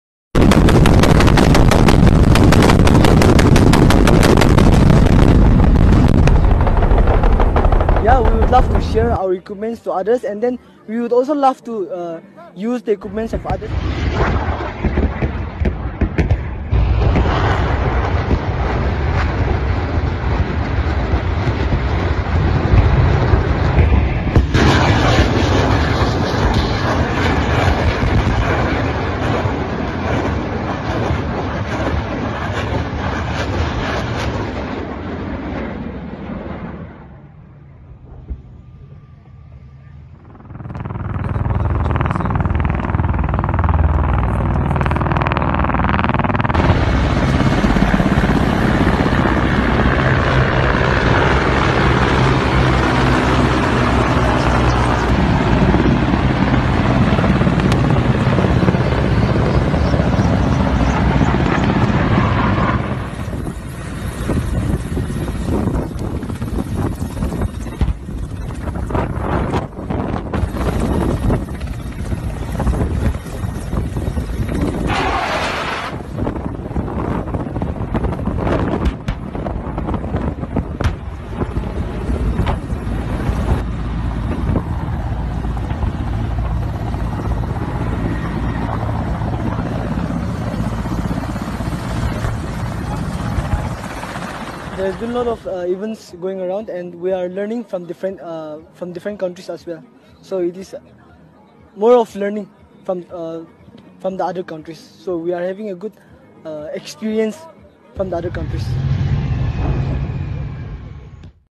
Naga Regiment: Visuals of Indian Army contingent participating in Multi Nation Exercise
Visuals of Indian Army contingent participating in Multi Nation Exercise ZAPAD2021.

Special Heliborne Operation SHBO were carried out as a joint rehearsal by Naga Regiment's Ghatak Platoon using MI-17 Helicopters.

Source: Indian Army Twitter Handle